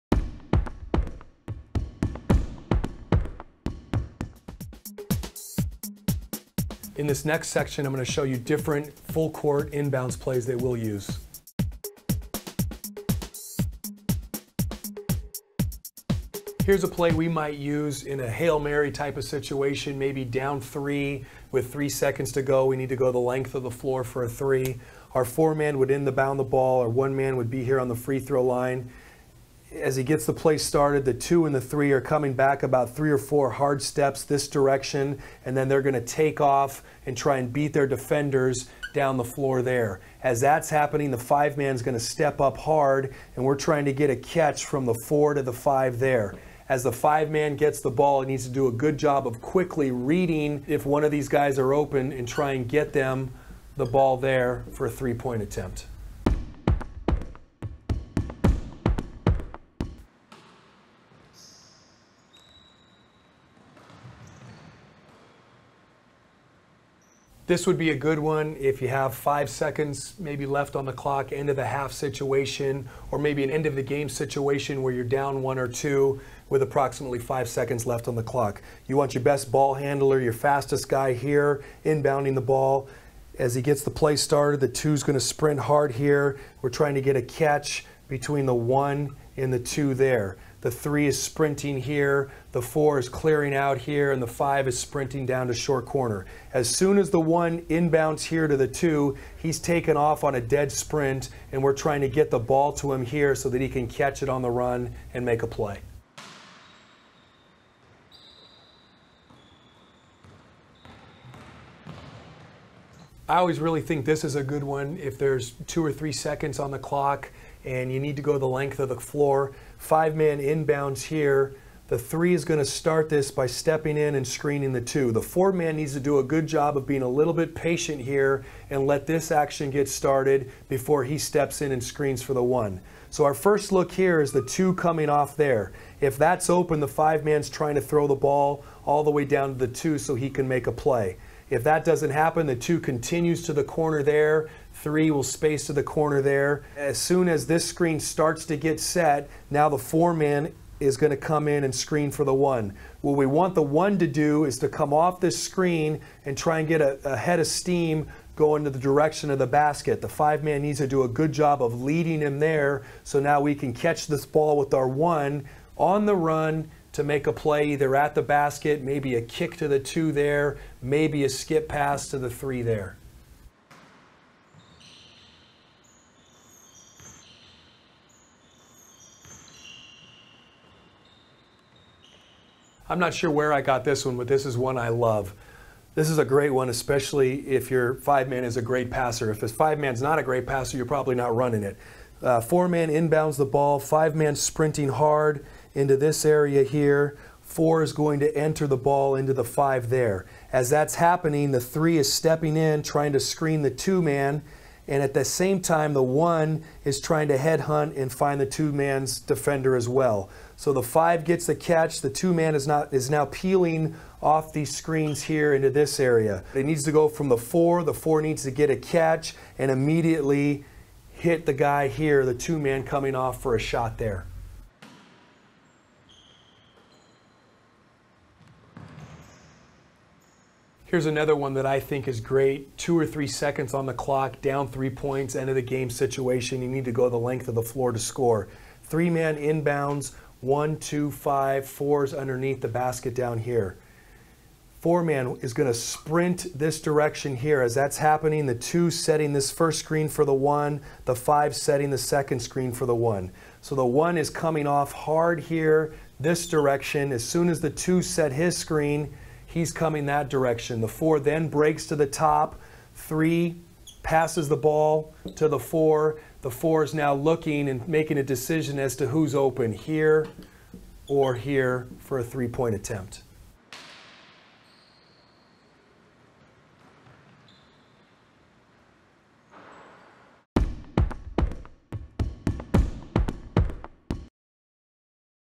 CrunchTime : In Bound Plays - Full Court Basketball Training Videos
because this DVD is a 'must have' addition to your arsenal of plays with over 16 scoring actions from a sideline out of bounds plays, almost 10 from baseline inbounds situations, 5 full court plays and a handful of special actions involving free throws and advancing the ball for a timeout.

Also Checkout for different Basketball coaching DVDs by Coaches around the world and Dominate the Game.